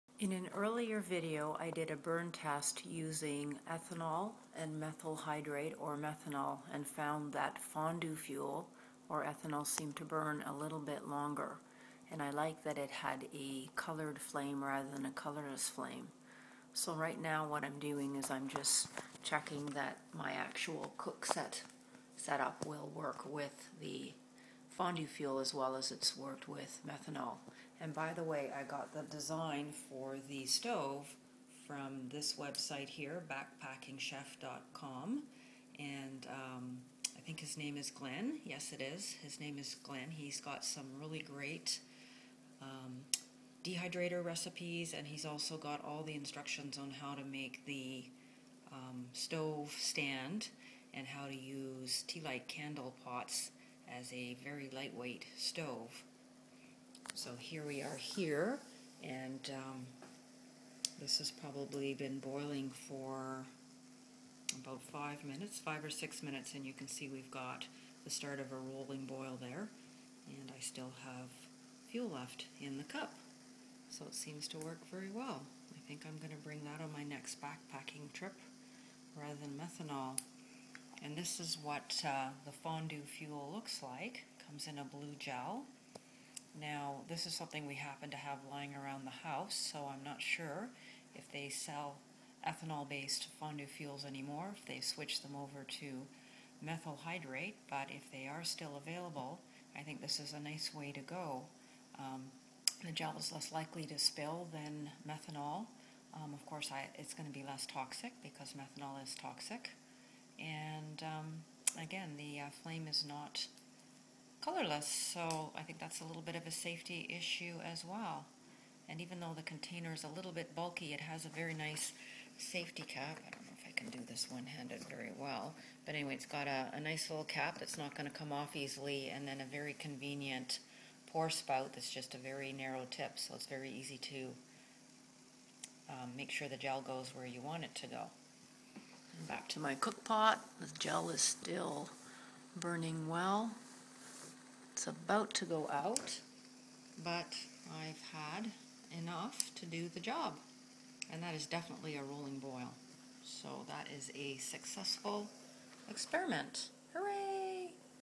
Fondue Fuel for Alcohol Backpacking Stove II
Burn Test using Chef Glen's Tealight Stove Design and Ethanol Fondue Fuel